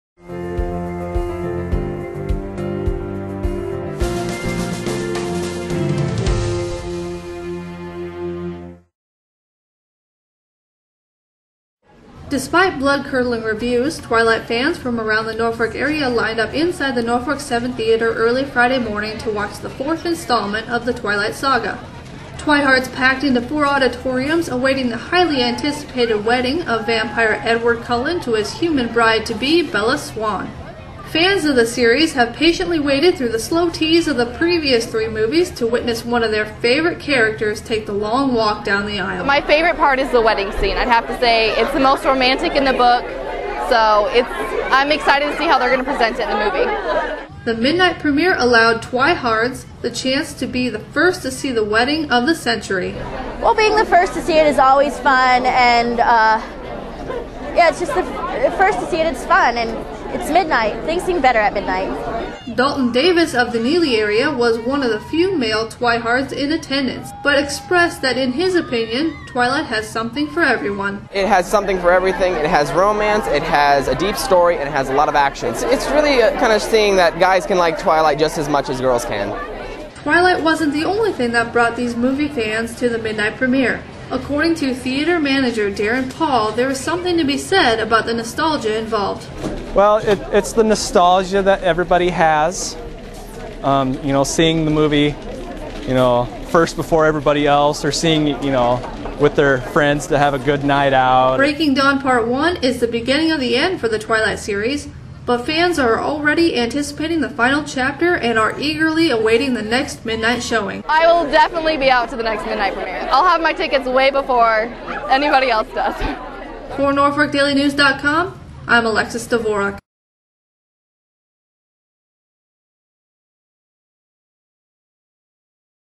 Twilight at midnight.wmv
Norfolk 7 premiers "Breaking Dawn - Part 1" at midnight.